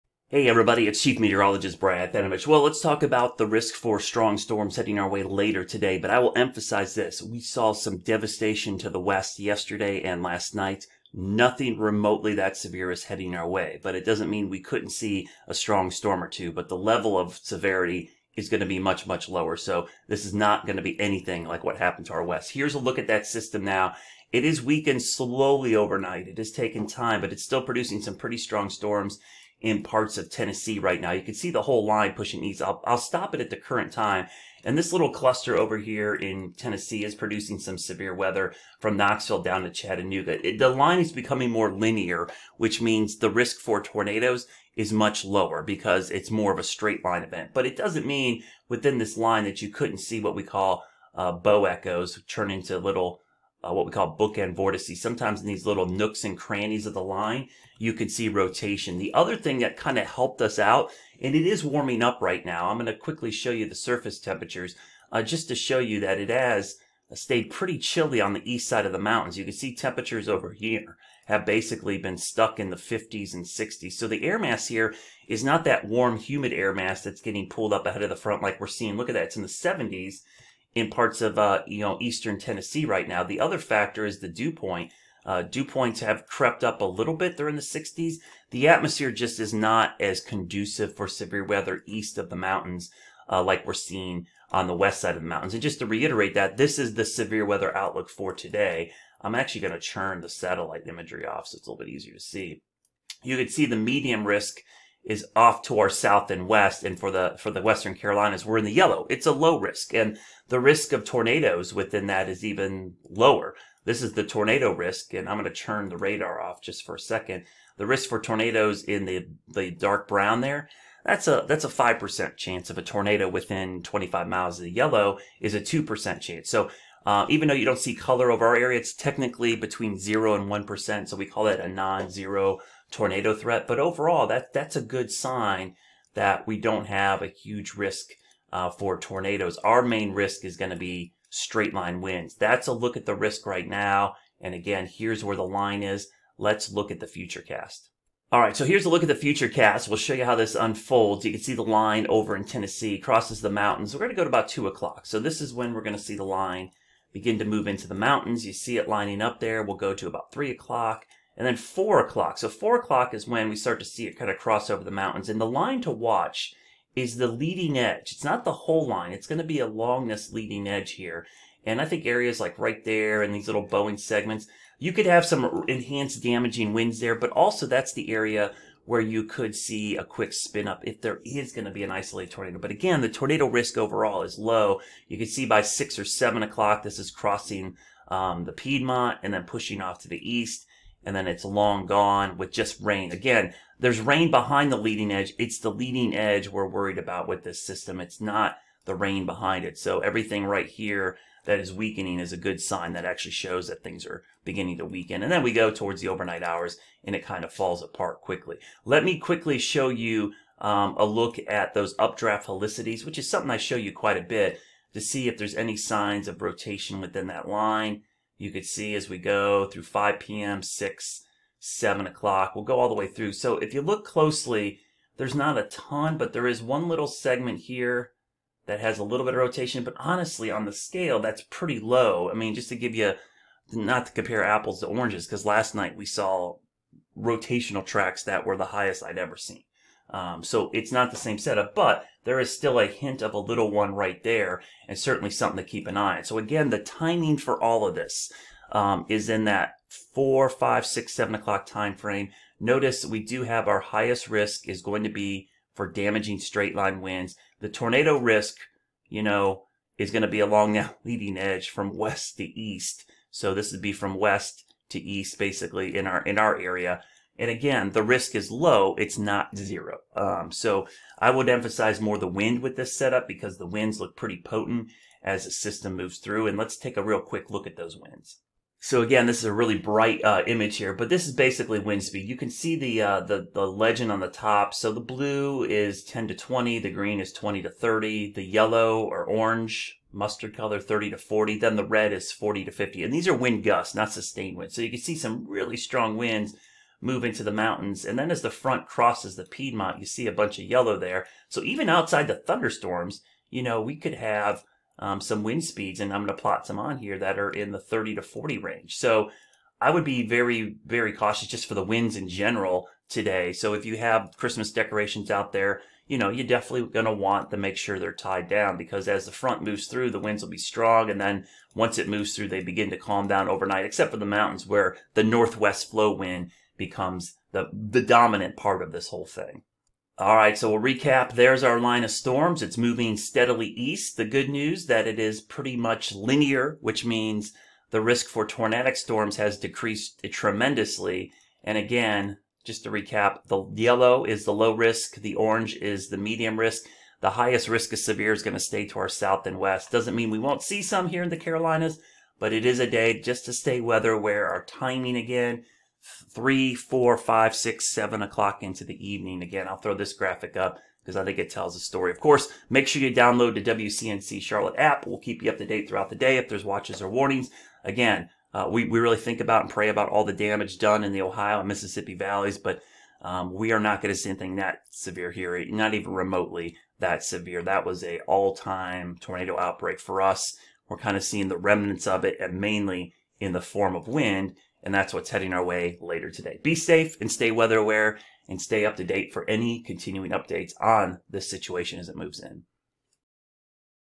Saturday WX VLOG 12/11/2021: Strong storms are possible but the risk is low this evening.
Saturday WX VLOG 12/11/2021: Strong storms are possible but the risk is low this evening. Our main concern is damaging straight-line winds but still, an isolated tornado can't be ruled out.